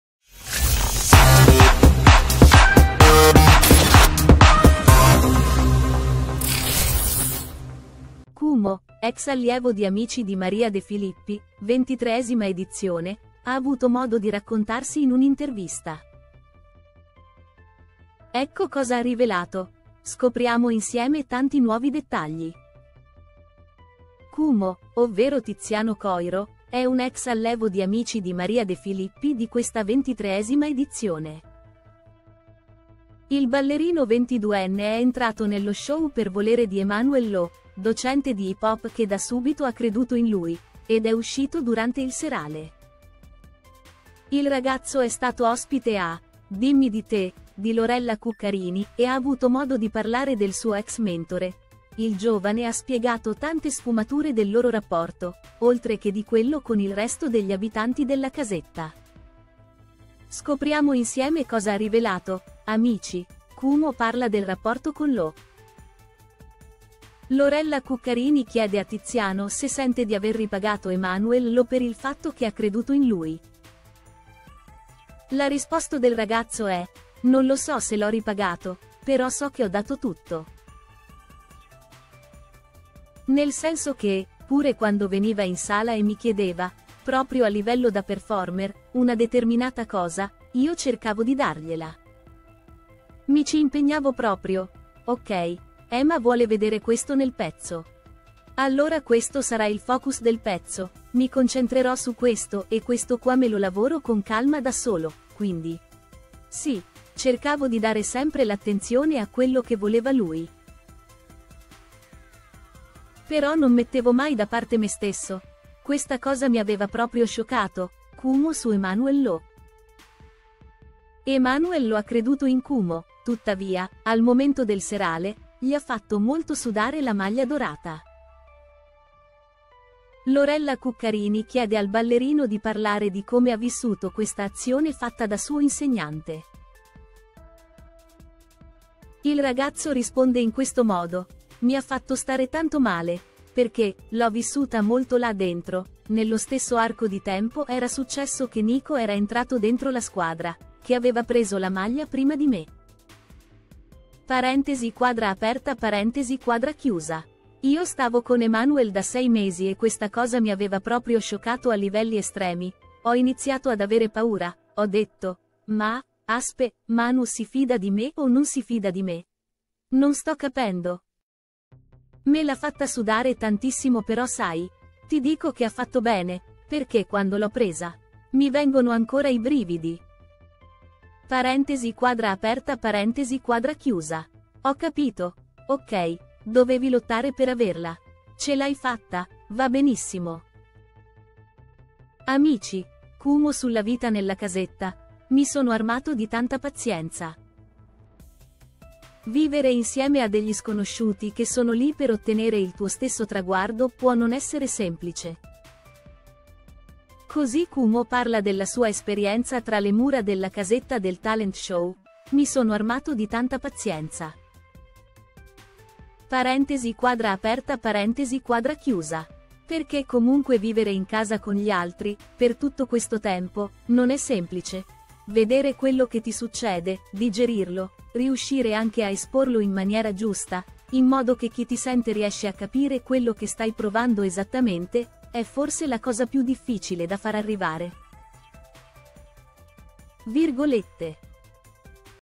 Amici, Kumo su Emanuel Lo “Mi ha fatto stare tanto male”
Kumo, ex allievo di Amici di Maria De Filippi, ventitreesima edizione, ha avuto modo di raccontarsi in un’intervista. Ecco cosa ha rivelato: scopriamo insieme tanti nuovi dettagli!
Kumo, ovvero Tiziano Coiro, è un ex allevo di Amici di Maria De Filippi di questa ventitreesima edizione. Il ballerino ventiduenne è entrato nello show per volere di Emanuel Lo, docente di hip hop che da subito ha creduto in lui, ed è uscito durante il serale. Il ragazzo è stato ospite a “Dimmi di te” di Lorella Cuccarini e ha avuto modo di parlare del suo ex mentore: il giovane ha spiegato tante sfumature del loro rapporto, oltre che di quello con il resto degli abitanti della casetta. Scopriamo insieme cosa ha rivelato!